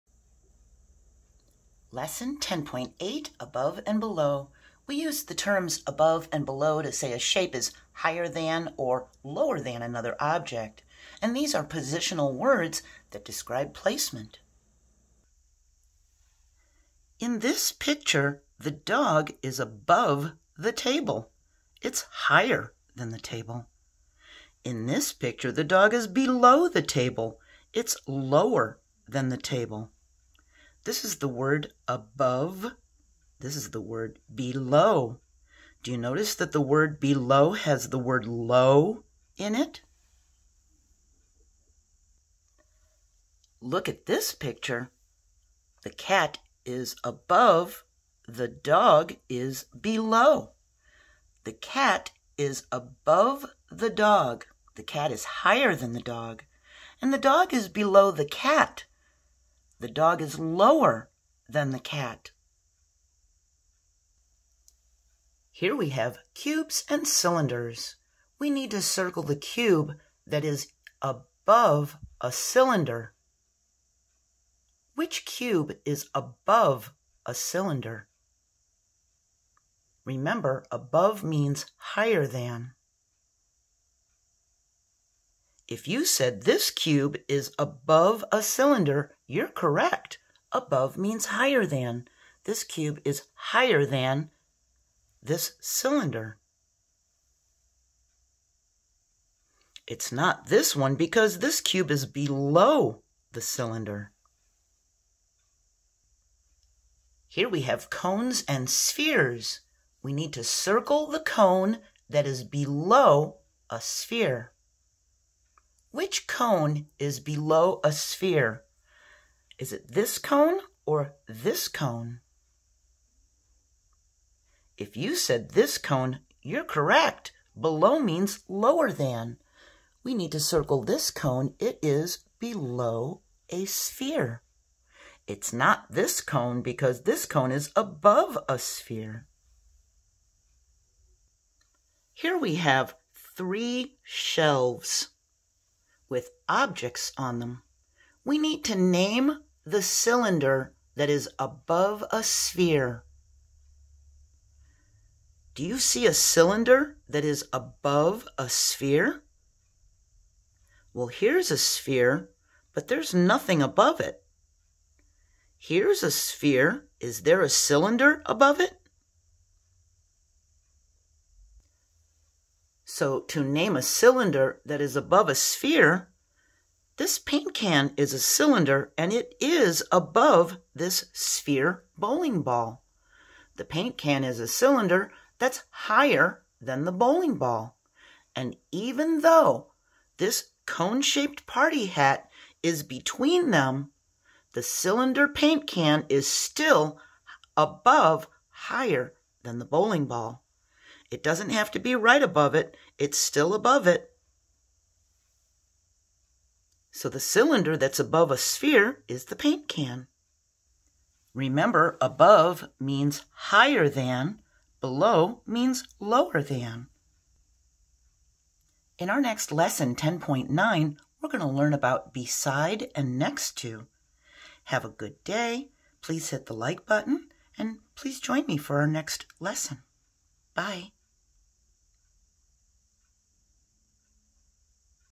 Kindergarten Math 10.8, Above and Below
We use the terms above and below to say a shape is higher than or lower than another object. These are positional words that describe placement. We identify solid shapes that are above or below an object, to review previous lessons on solid shapes.